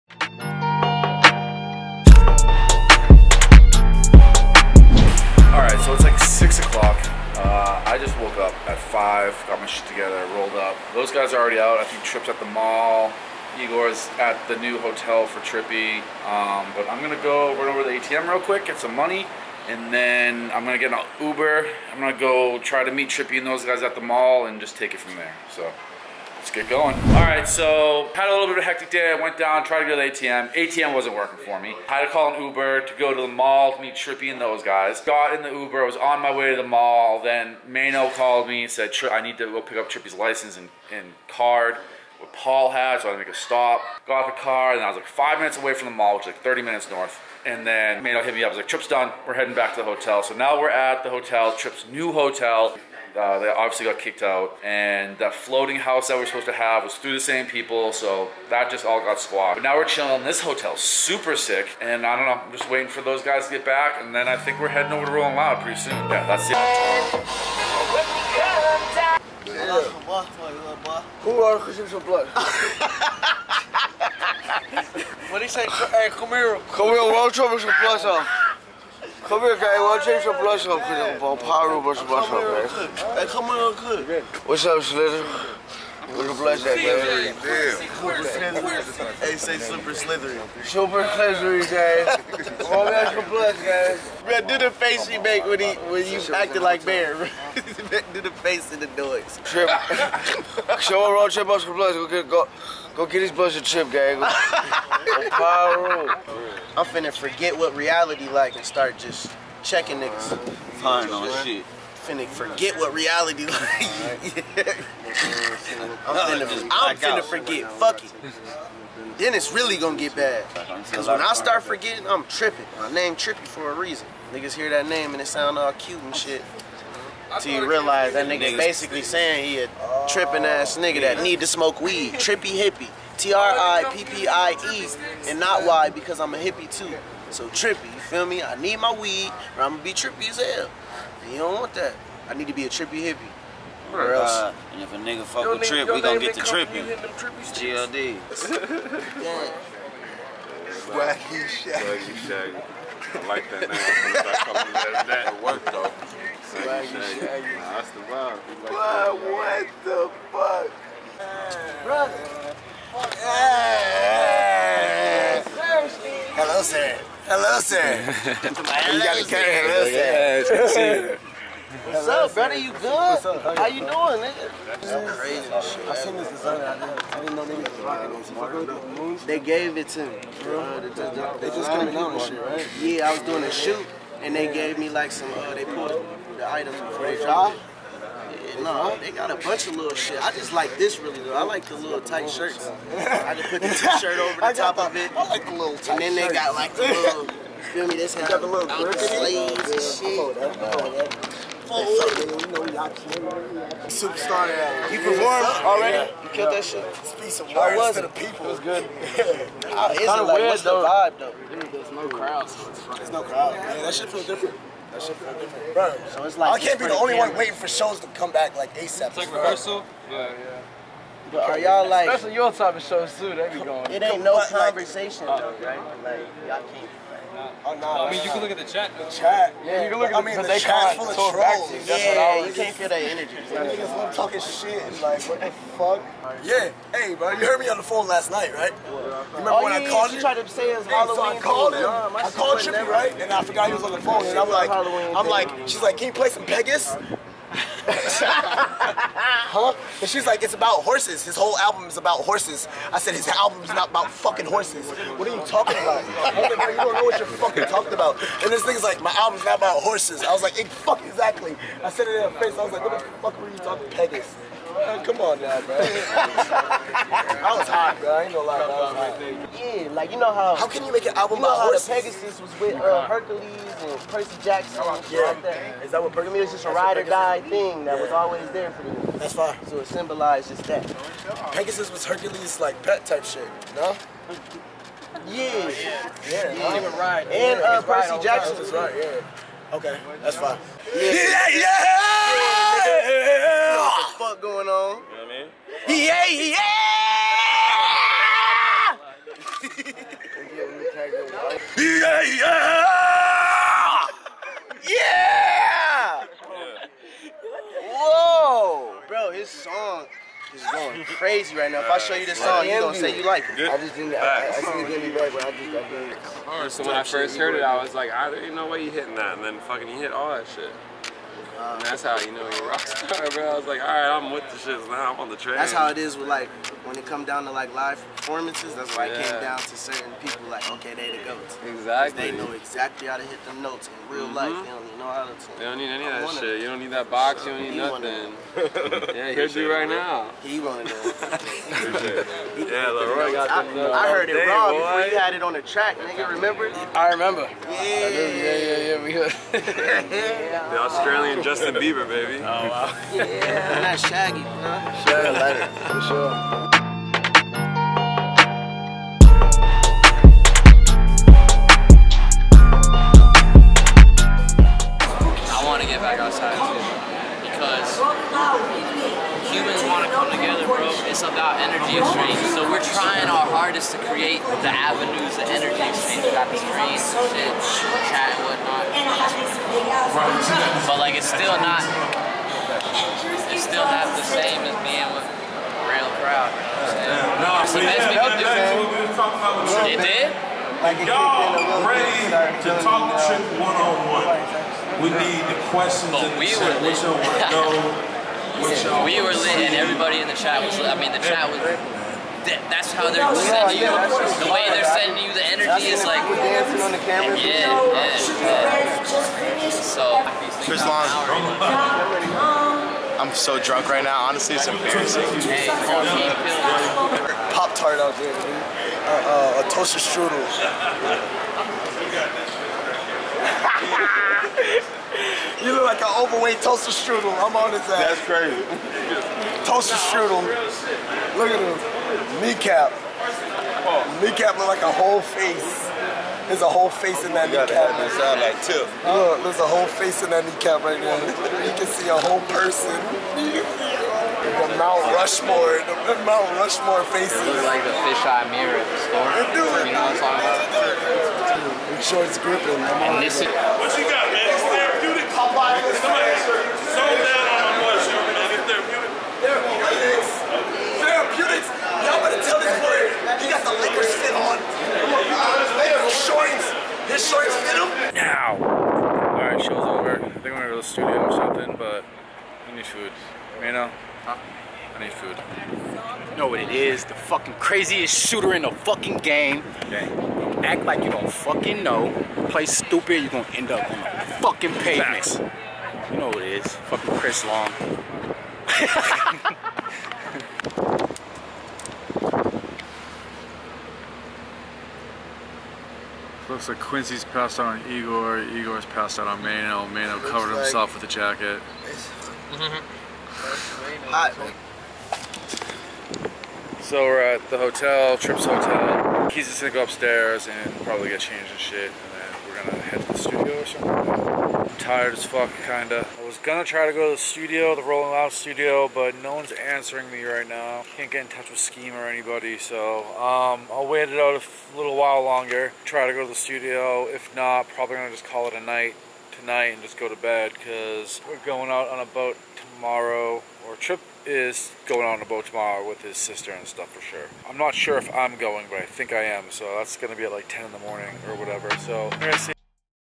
Rolling Loud with Trippie Redd [Miami part 2]
In part 2 of my trip to Miami with Trippie Redd we head over to Rolling Loud for trips performance. We ran into The Kid Laroi, Ski Mask The Slump God, Mario Judah & more at the event. Naturally we had our own little party in the parking lot before the show hanging out laughing smoking which is always the best part. It was a lot different being a live streamed show instead of having the crowds but, it was still a good time.